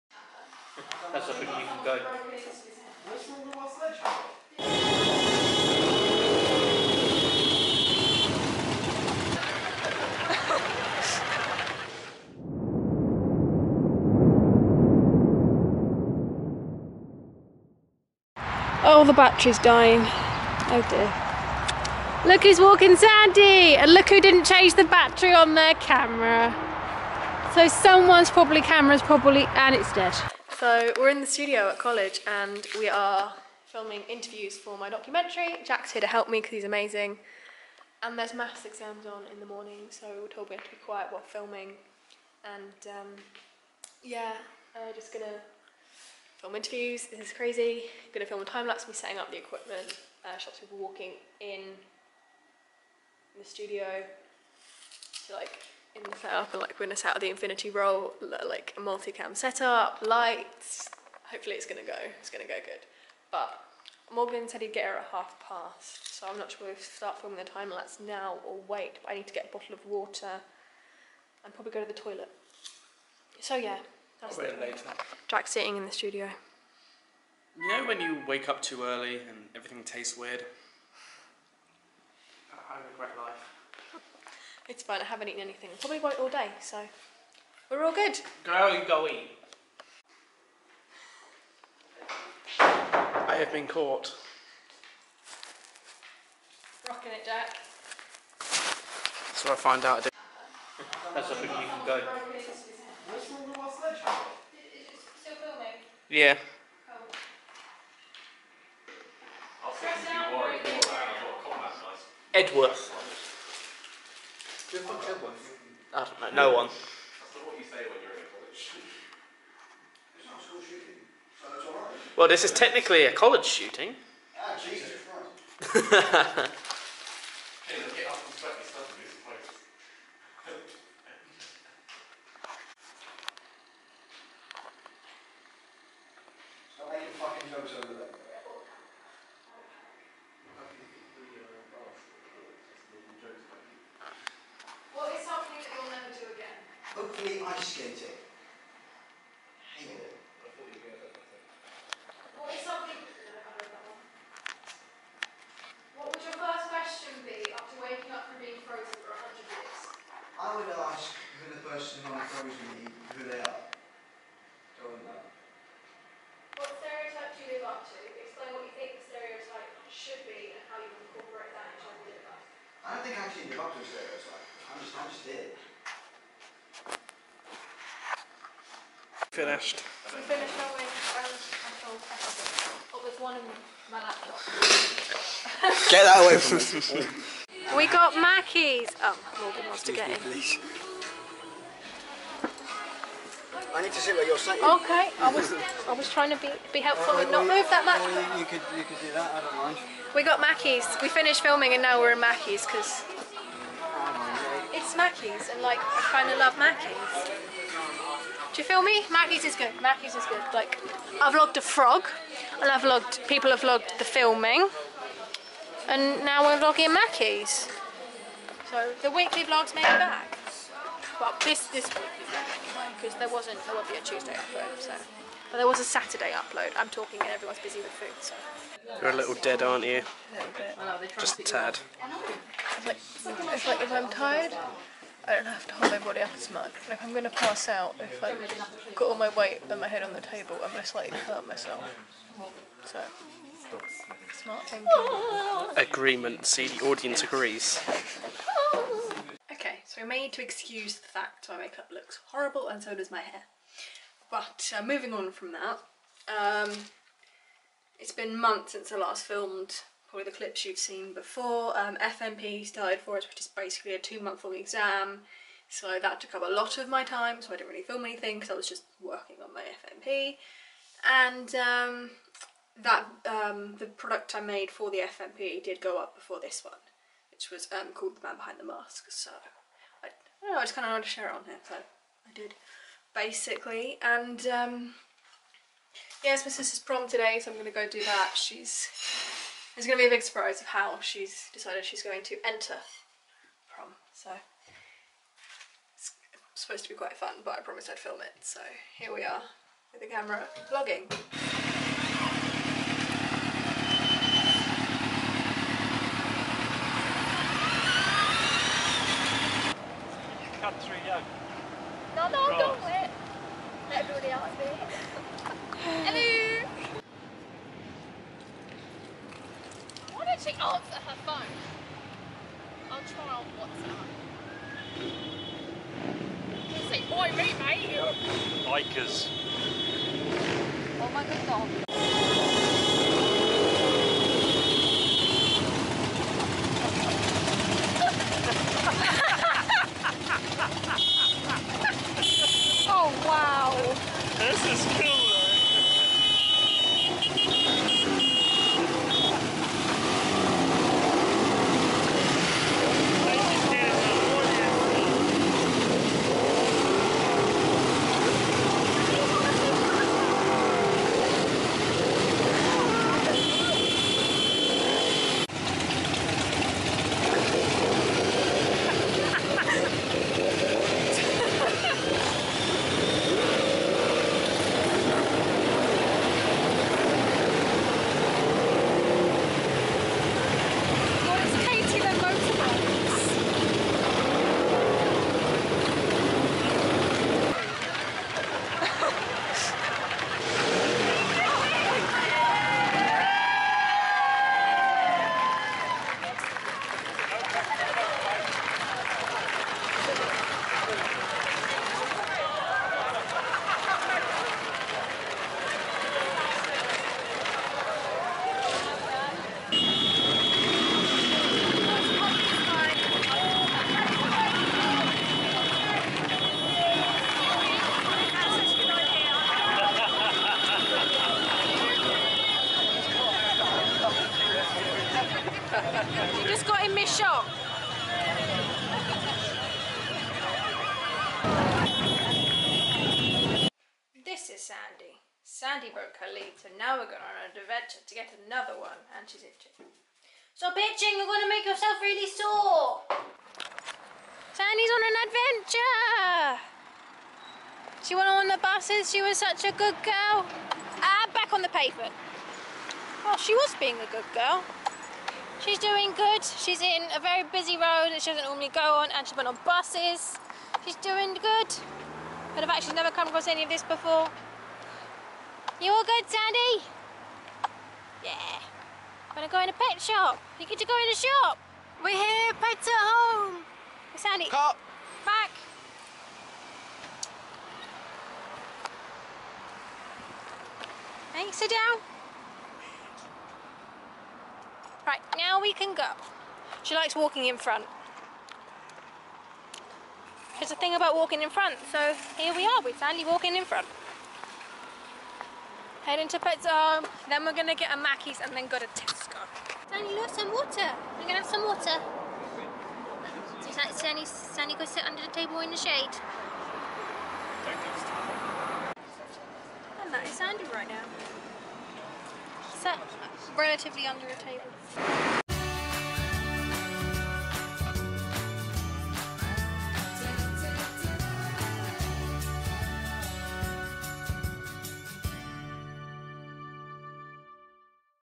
THE PROM ENTRANCE OF DREAMS
This video is a lot of random footage from my time during FMP and my sister's prom.